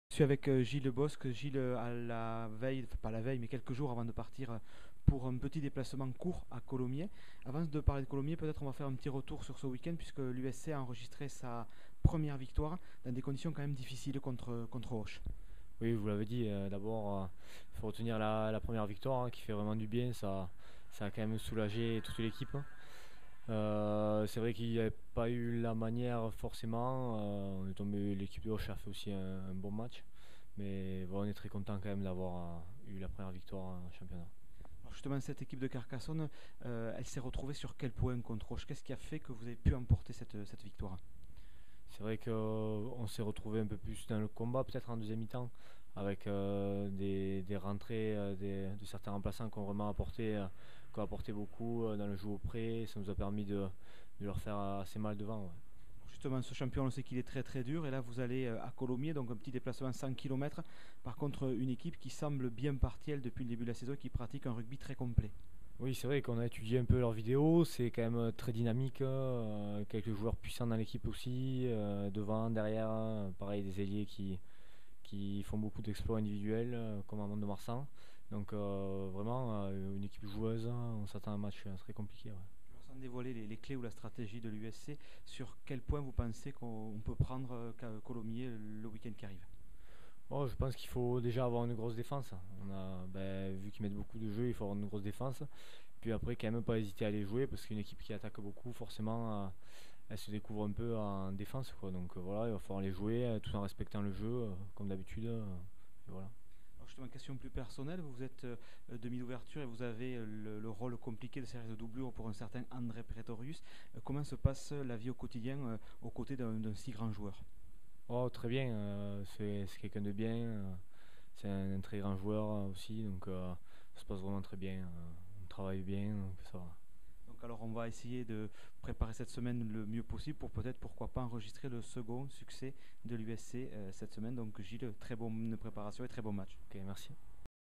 US CARCASSONNE : Gilles BOSCH avant le déplacement à Colomiers ( vidéo rugby )
Retrouvez Gilles BOSCH, le jeune demi d'ouverture de l'USC pour un retour sur la 1ère victoire contre Auch et la préparation du déplacement à Colomiers pour le compte de la 4ème journée de ProD2.

Propos recueillis par Valérian GALLET et Patrick FASSINA